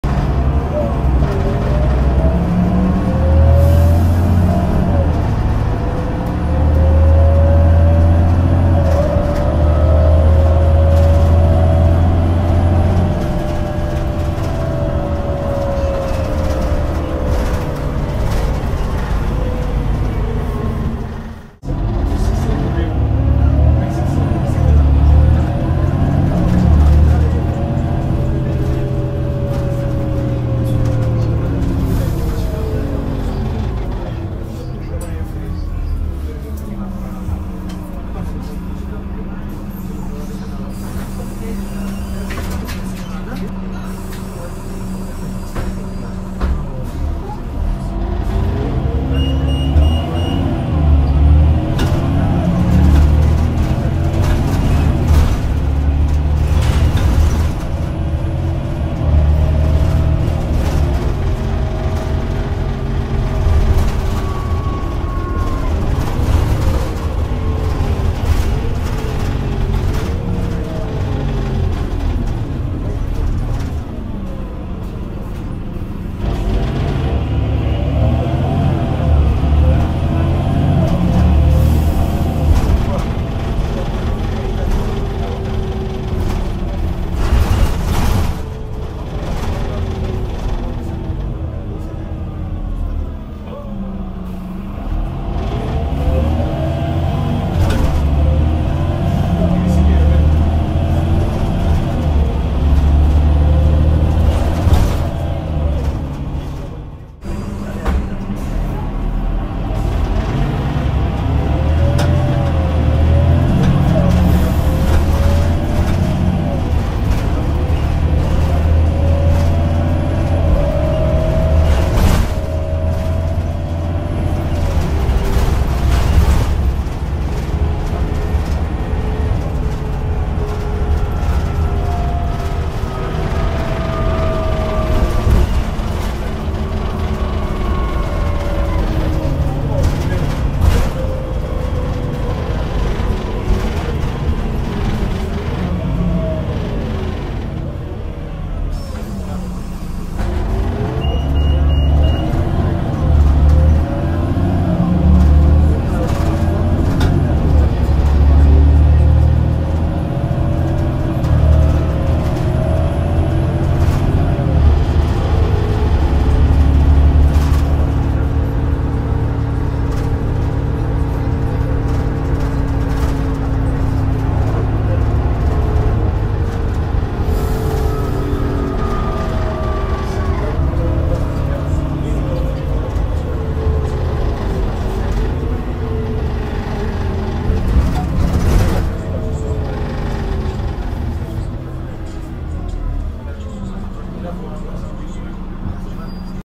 🎙️[RATP] IrisBus Citelis Line {3497} ZF 6HP-502C [Full Kickdown] !!!!!!
« Habillage Réformé »

Châssis : Citelis
Moteur : Iveco Cursor 8 180kW-245CH
Transmission : ZF Ecomat 6HP502C
Mise en service : 2009
Ligne D'affectation : 111
Centre Bus : St Maur 💥

Ecouteurs/Casques ou Enceintes recommandé sur ce bonne écoute.